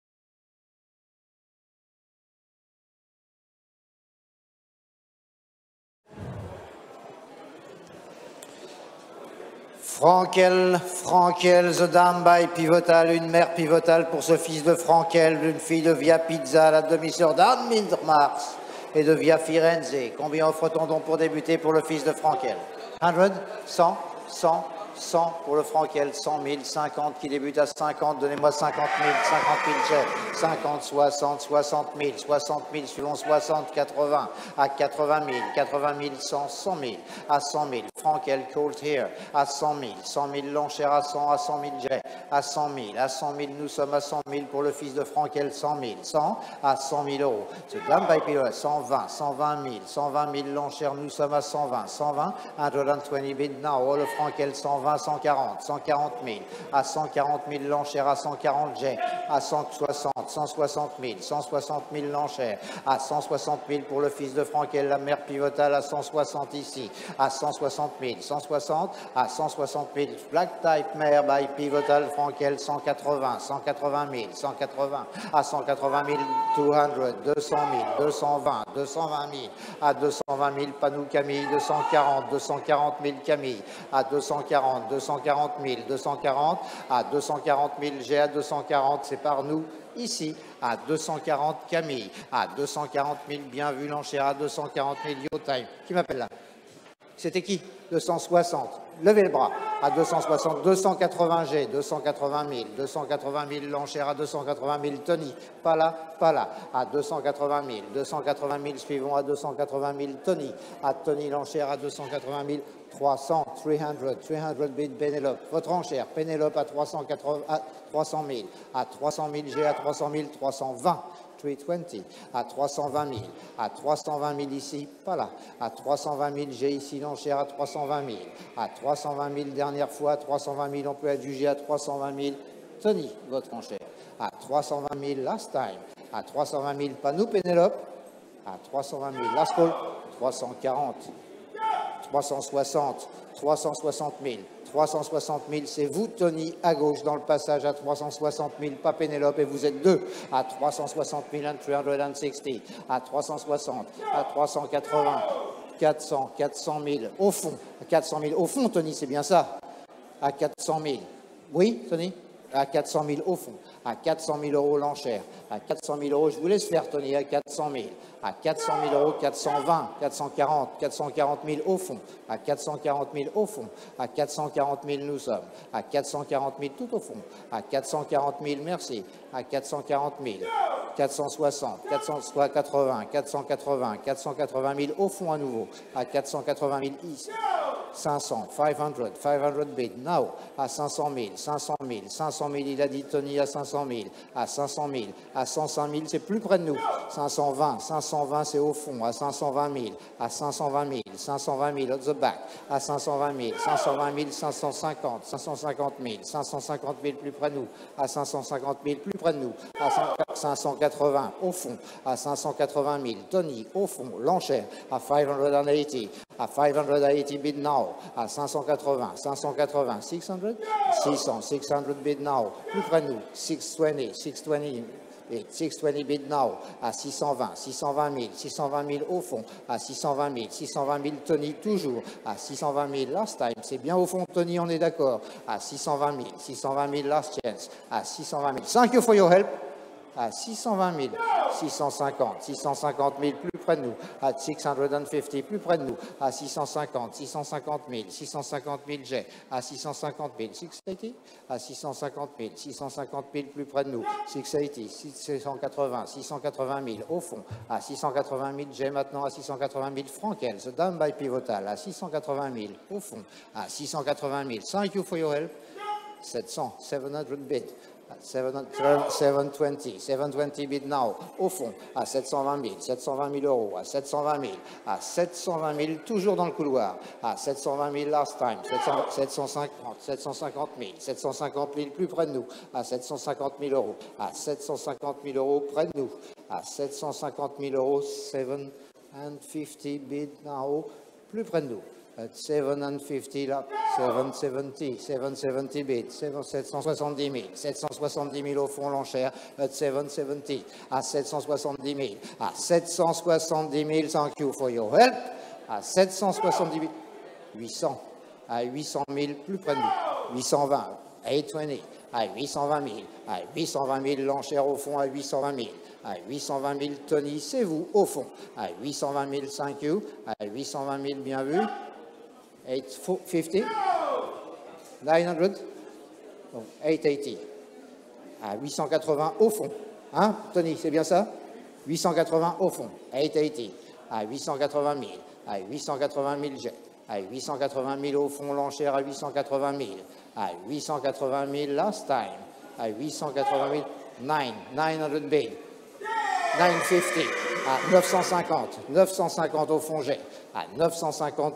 Top Price J1 - N°2 : 950 000E - N (VIA PISA 2022) - Vente Arqana d'Août 2023
Vente Arqana d'Août 2023
Top Price J1 - N°2 - Lot 111
N (VIA PISA 2022)
M22 FRANKEL x VIA PISA
Saint Pair - 950 000E